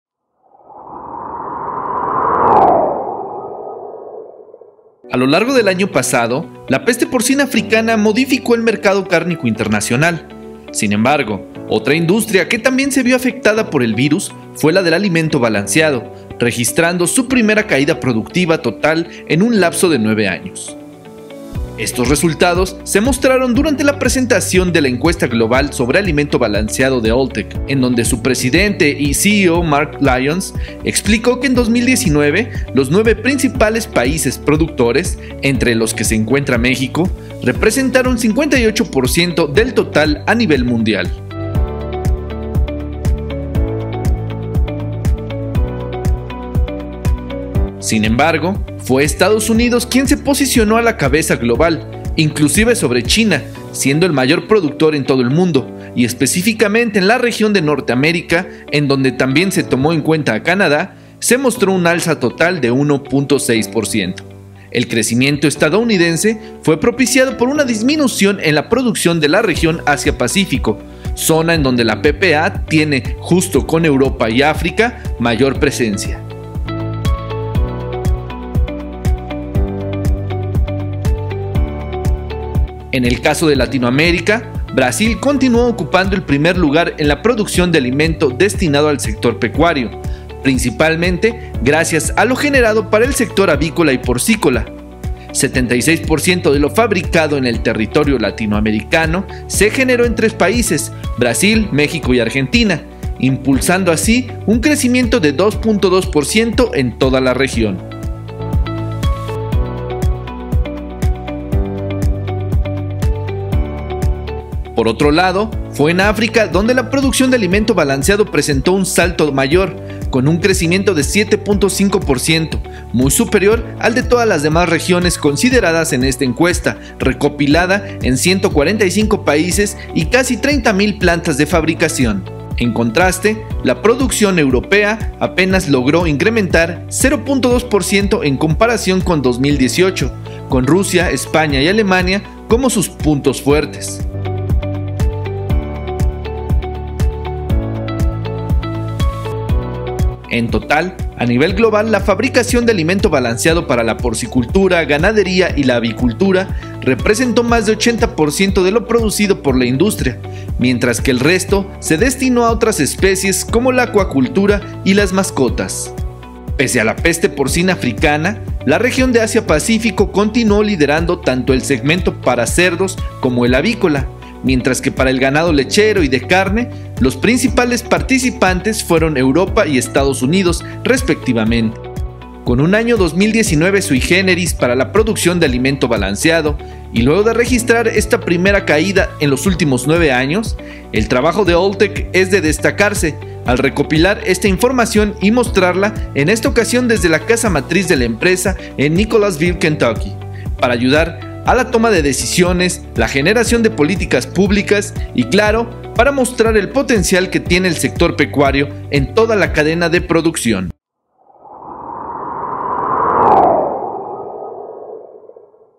La producción mundial de alimento balanceado tuvo su primer caída en nueve años
Alltech publicó los resultados de su Encuesta Global sobre Alimento Balanceado en su edición 2020, en ella se informó que la producción mundial cayó 1.07%, el primer resultado a la baja luego de nueve años. Se cerró 2019 con un total de 1,126 millones de toneladas métricas.

La producción de alimento balanceado es una industria que va de la mano con el sector pecuario, por lo que si una se ve afectada, impacta directamente en la otra; la situación que vive la región Asia - Pacífico desde 2018 por el tema de la Peste Porcina Africana, su sector cárnico de porcino, y otros factores como las relaciones comerciales, reflejan la volatilidad en esta cifra a la baja.